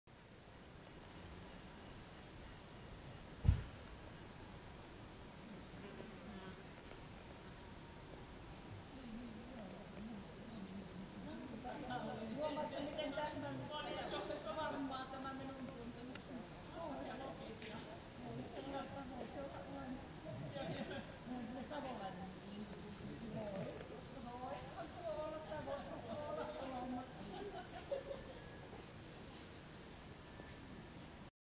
On the footpath, I meet an interesting insect
On the footpath, I meet an insect (Diptera) waving its front legs.

Location: Savonlinna, Finland.

Polulla tuli vastaan etujalkojaan heilutteleva hyönteinen (Diptera).